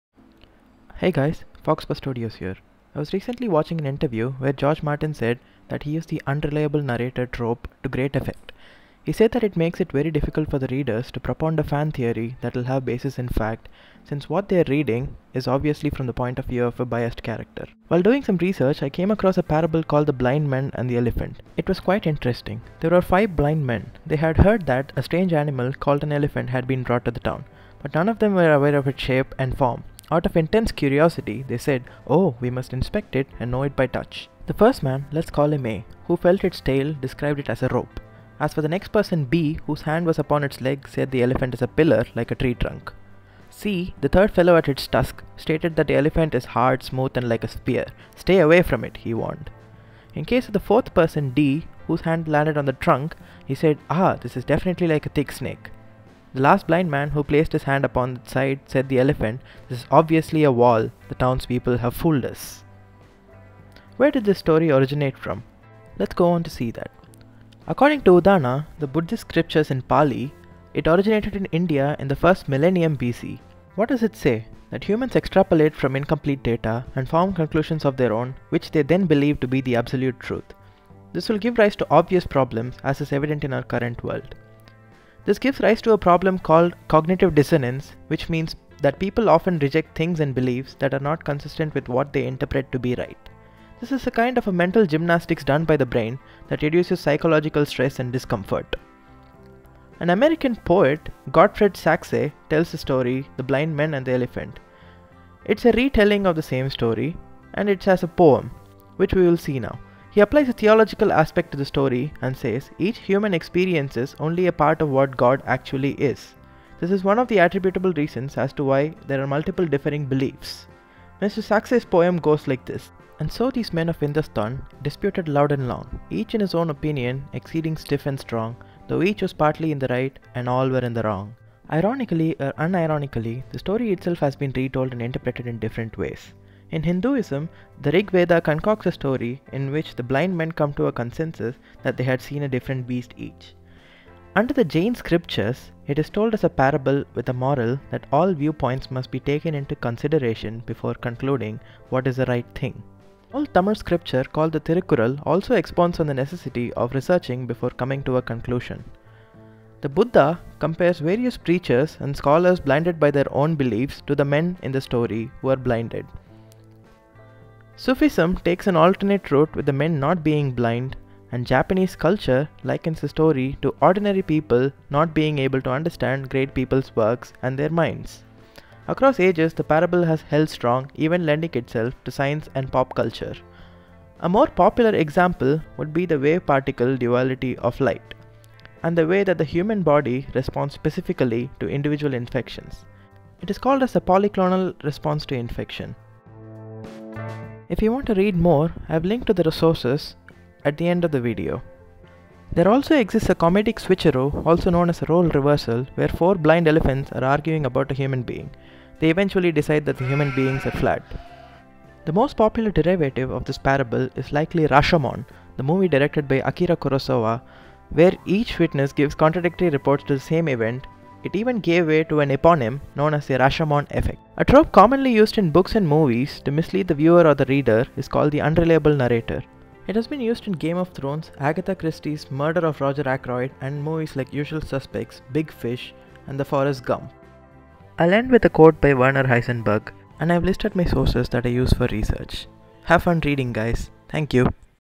Parables - The Blind Men and The Elephant | iPad | iMovie | Procreate | Adobe Spark
Have you ever wondered what cognitive dissonance is? Or have noticed that people usually don't believe beyond their faiths?

This video is a look into an age old tale and its influence on the world, modern and ancient.

Made with Procreate and iMovie, with Adobe Spark to help.

Inspired by Casually Explained, @DrawWithJazza, Armando Hasudungan and Osmosis.